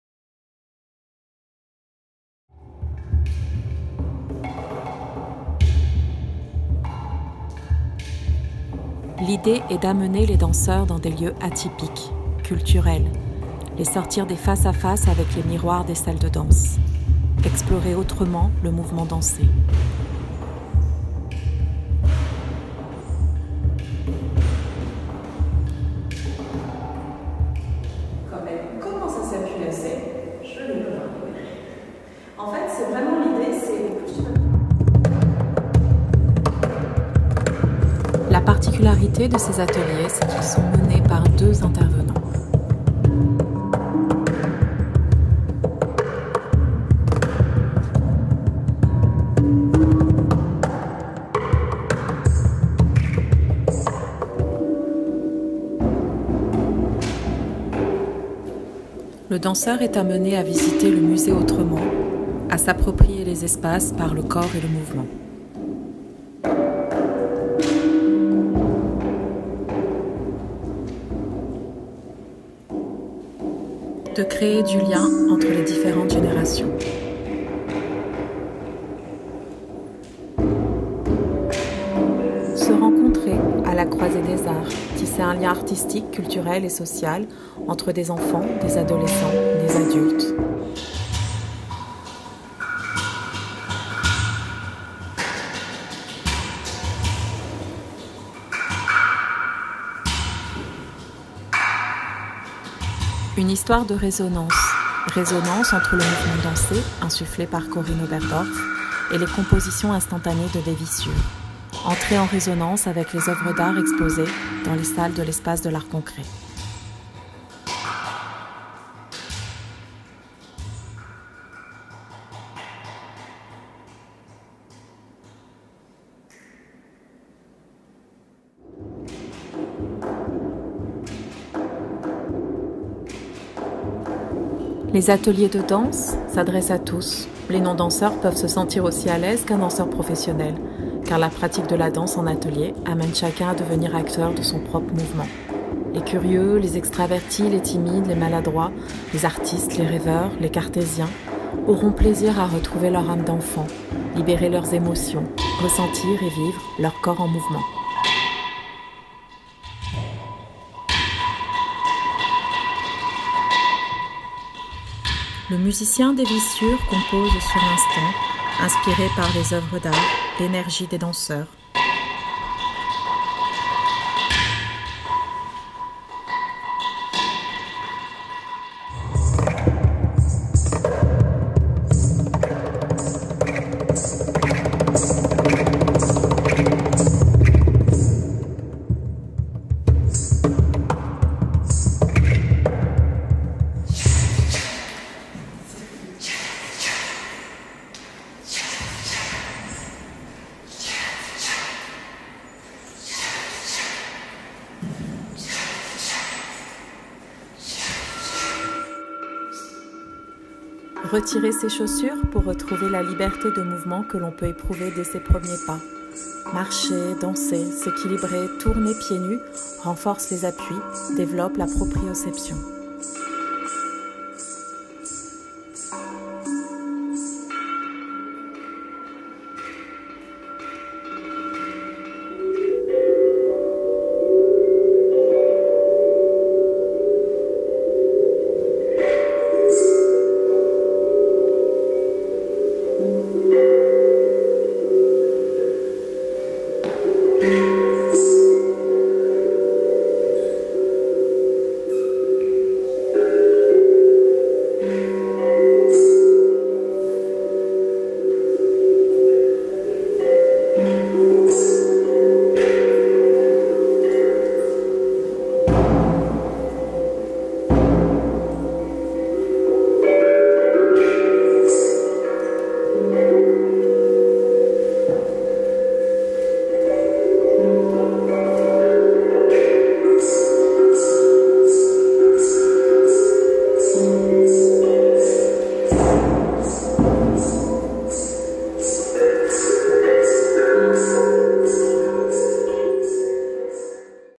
Danse & Musique à l'Espace de l'Art Concret
documentaire : Ateliers danse & musique instantanée tous publics à l'Espace de l'Art Concret (Mouans-Sartoux-06 FR)